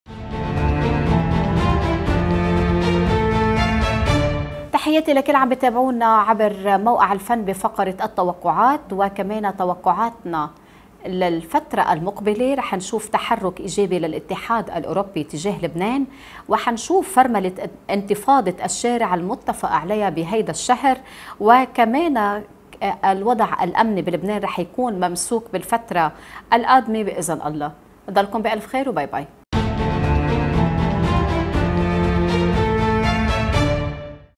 بالذكرى الأولى لفاجعة بيروت.. جومانا وهبي تكشف تطورات أمنية مفاجئة
Copyright : ElFann.com